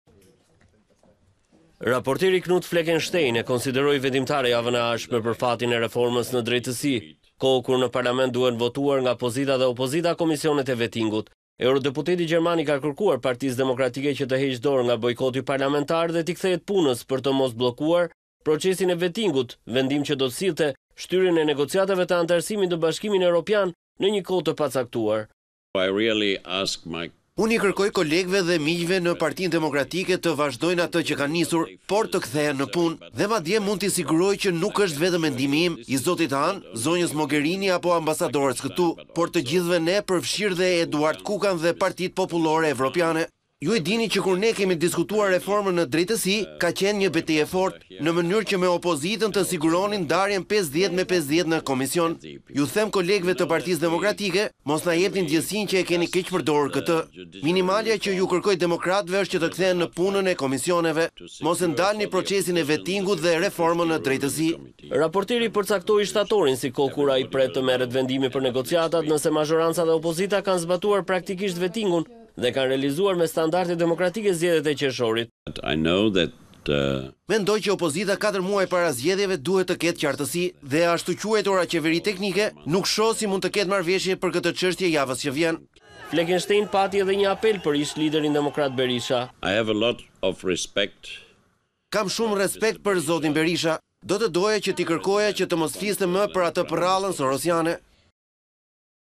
Thirrja e Fleckenstein: Kthehuni! Mos ndaloni Vettingun - Top Channel Albania - News - Lajme
Raporteri Knut Fleckenstein e konsideroi vendimtare javën e ardhshme në Parlament për reformën në drejtësi, kohë kur duhen votuar nga opozita dhe pozita komisionet e Vettingut.